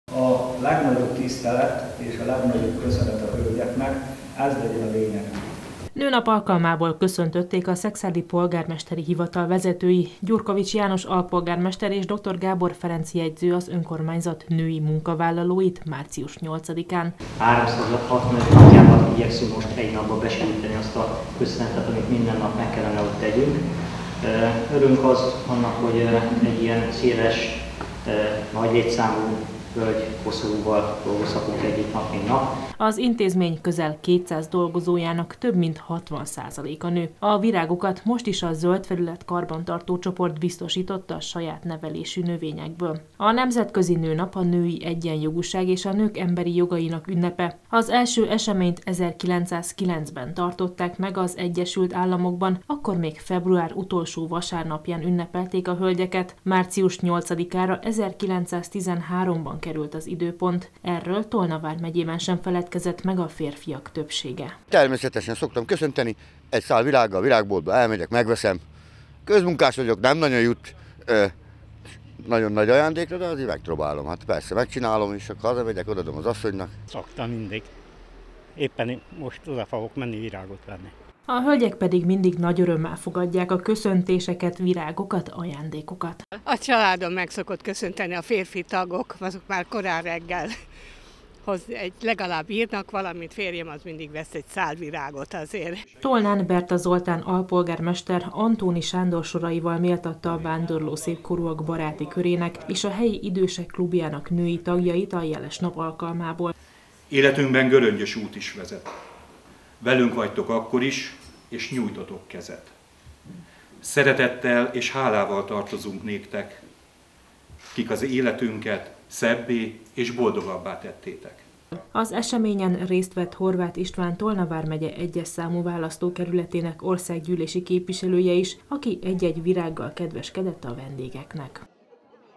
Nemzetközi nőnap - a tisztelet és megbecsülés kifejezésének eseménye
A nemzetközi nőnap a hölgyek iránti tisztelet és megbecsülés kifejezésének napja, amelyet 1917 óta minden év március 8-án világszerte ünnepelnek. Tolna vármegyében is sokan gondolták úgy, hogy  ilyenkor fontos meglepni a környezetükben elő nőket.